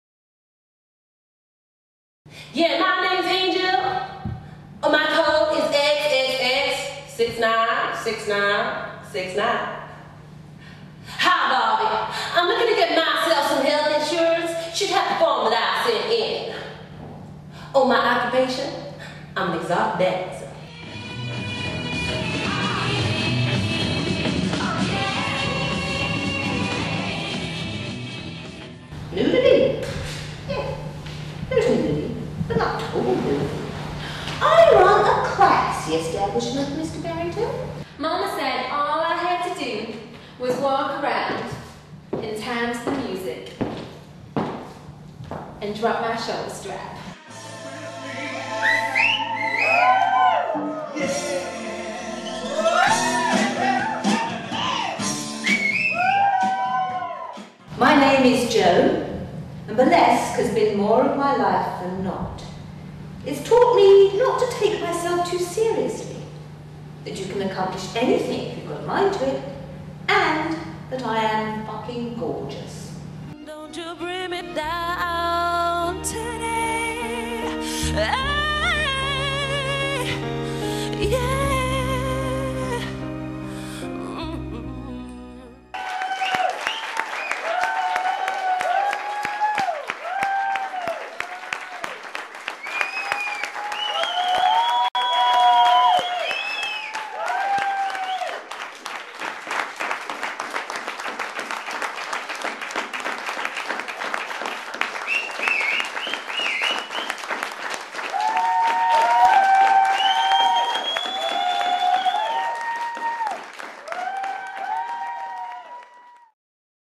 Burlexe: Original Trailer
Burlexe is a new theatrical project about the stories of the women who've created burlesque.
Burlexe stars popstar Javine Hylton and la creme de la creme of the UK's burlesque scene including Jo King, Kiki Kaboom and Fancy Chance together with actresses Chloe Ewart and Dympna Messenger.